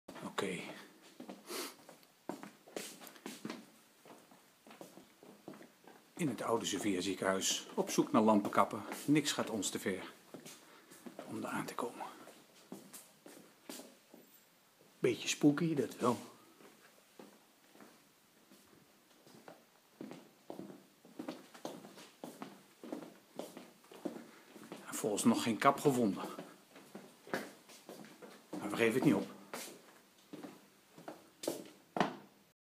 Zoektocht lampenkappen Sassenstraat Allicht Sofia Ziekenhuis deel 1
Lampenkappen gezocht! Na een tip van een meneer uit Zwolle hebben we contact opgenomen met de uitvoerder van het oude Sofia Ziekenhuis in Zwolle. Daar moesten nog heel veel lampenkappen liggen. Dit filmpje laat zien hoe Tom en Marc de gangen afstruinen, tot nu toe niets gevonden. Uiteindelijke wel, een hele berg.